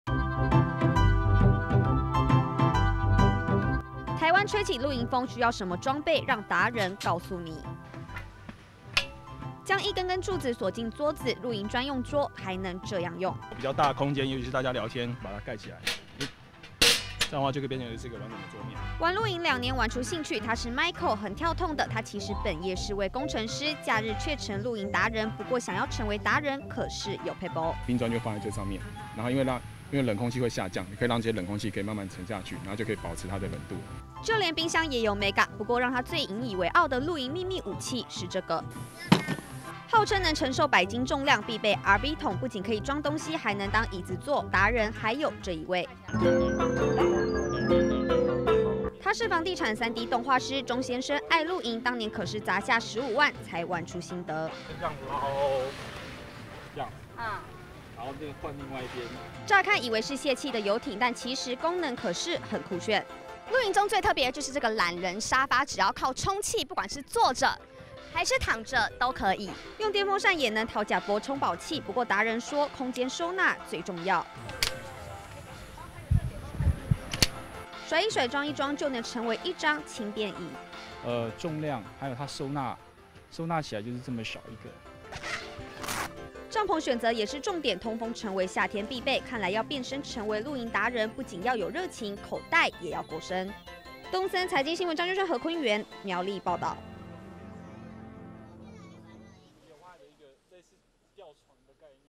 科技新貴砸10萬變露營達人!收納空間最重要
台灣人瘋露營，不過要成為達人要什麼裝備？有一位科技工程師玩露營兩年，就連攜帶桌子跟冰箱擺放，都有撇步！另外一位動畫師，可是砸下15萬露營超過百次，他說收納空間最重要，我們帶您來看！